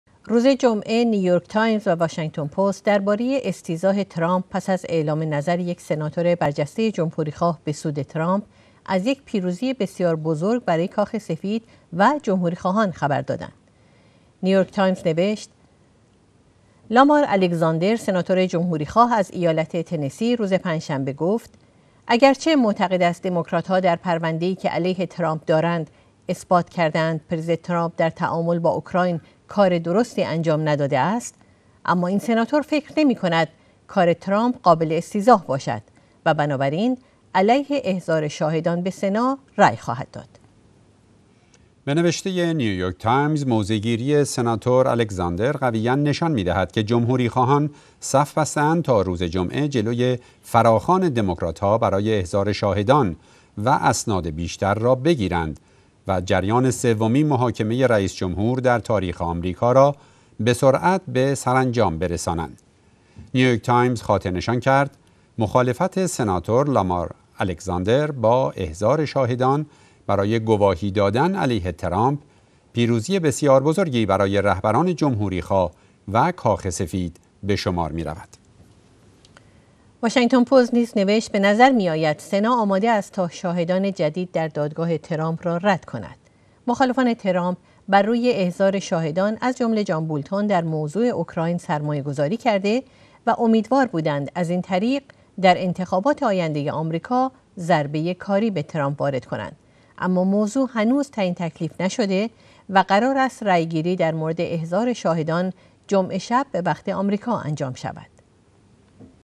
نیویورک تایمز و واشینگتن پست درباره استیضاح ترامپ، از یک پیروزی بسیار بزرگ برای کاخ سفید و جمهوریخوا
نیویورک تایمز و واشنگتن پست درباره استیضاح ترامپ، از یک پیروزی بسیار بزرگ برای کاخ سفید و جمهوریخواهان خبردادند
روز جمعه نیویورک تایمز و واشنگتن پست درباره استیضاح ترامپ، پس از اعلام نظر یک سناتور برجسته جمهوریخواه به سود ترامپ،  از یک پیروزی بسیار بزرگ برای کاخ سفید و جمهوریخواهان خبردادند.
نیویورک تایمز نوشت:
 لامار الگزاندر سناتور جمهوریخواه از ایالت تنسی روز پنجشنبه گفت
 اگر چه معتقد است دموکراتها در پرونده ای که علیه ترامپ دارند اثبات کرده اند پرزیدنت ترامپ در تعامل با اوکراین کار درستی انجام نداده است،
 اما این سناتور فکر نمی کند کار ترامپ قابل استیضاح باشد و بنابراین علیه احضار شاهدان به سنا رأی خواهد داد.
به نوشته نیویورک تایمز:
 موضعگیری سناتور الگزاندر قویاً نشان می دهد که جمهوریخواهان صف بسته اند تا روز جمعه جلوی فراخوان دموکراتها برای احضار شاهدان و اسناد بیشتر را بگیرند
 و جریان سومین محاکمه رییس جمهور در تاریخ آمریکا را بسرعت به سرانجام برسانند.
نیویورک تایمز خاطر نشان کرد :
مخالفت سناتور لامار الگزاندر با احضار شاهدان برای گواهی دادن علیه ترامپ ، پیروزی بسیار بزرگی برای رهبران جمهوریخواه و کاخ سفید بشمار می رود.
واشنگتن‌پست نیز نوشت:
به نظر میاید سنا آماده است تا شاهدان جدید در دادگاه ترامپ را رد کند.
مخالفان ترامپ بر روی احضار شاهدان از جمله جان بولتون در موضوع اوکراین سرمایه گذاری کرده و امیدوار بودند از این طریق در انتخابات آینده آمریکا ضربه کاری به ترامپ وارد کنند.
 اما موضوع هنوز تعیین تکلیف نشده و قرار است رای گیری در مورد احضار شاهدان جمعه شب به وقت آمریکا انجام شود.